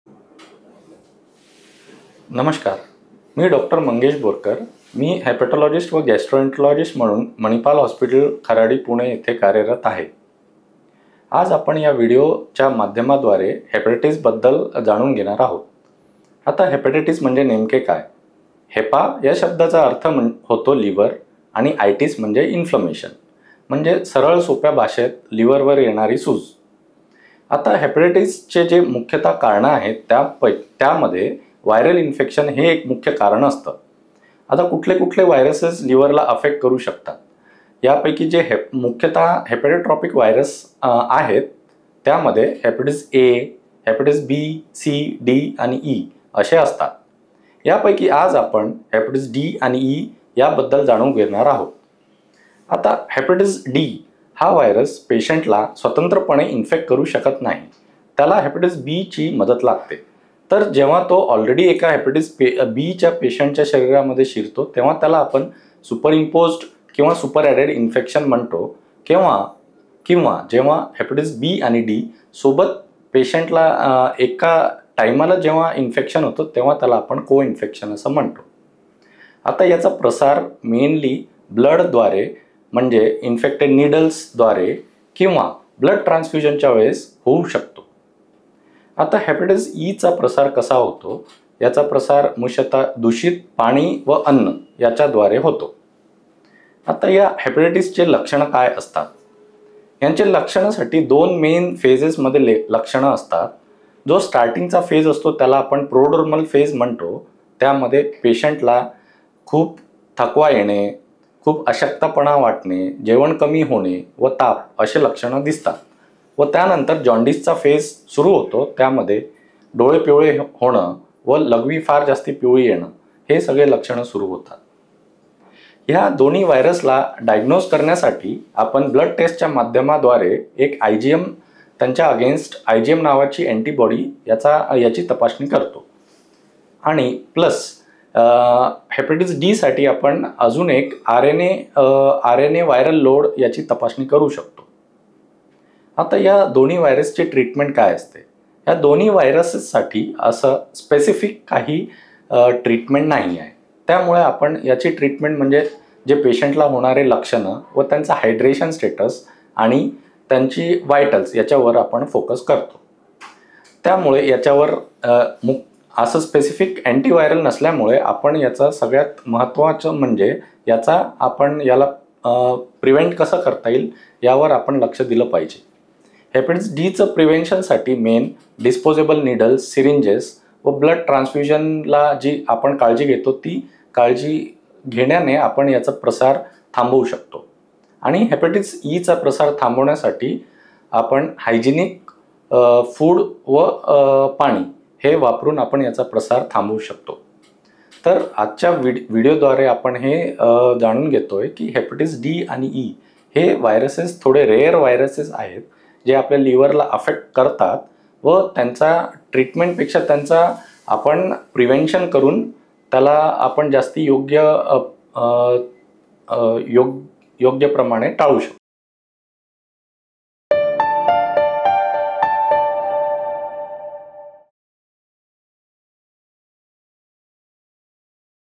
World Hepatitis Day:हिपॅटायटीसचे प्रकार आणि संक्रमण मार्ग माहीत आहेत का? जाणून घ्या तज्ज्ञांचा सल्ला
२८ जुलै दिवस जागतिक हिपॅटायटीस दिवस म्हणून साजरा केला जातो. हिपॅटायटीसबद्दल जागरुकतेचा अभाव ही मोठी समस्या आहे. लक्षणे दिसू लागतात तेव्हाच बहुतेक लोक डॉक्टरकडे जातात. त्यावेळी विषाणूने यकृतावर आधीच परिणाम केलेला असतो. अशा परिस्थितीत लक्षणांची माहिती आणि त्याविषयी जागरुकता जीव वाचवू शकते. हिपॅटायटीस म्हणजे काय आणि हिपॅटायटीस डी आणि ई बद्दल तज्ञ काय म्हणतात.  व्हायरल हिपॅटायटीसचे कमी-ज्ञात प्रकार  काय आहेत हे आपण जाणून घेऊयात या व्हिडिओच्या माध्यमातून डॉ मंगेश बोरकर यांच्याकडून. संसर्गाचा सामना करण्याचा सर्वात प्रभावी मार्ग म्हणजे तो ओळखणे. त्याची लक्षणे समजून घेऊन योग्य वेळी तपासणी करुन घेतल्यास रुग्णाचा जीव वाचू शकतो.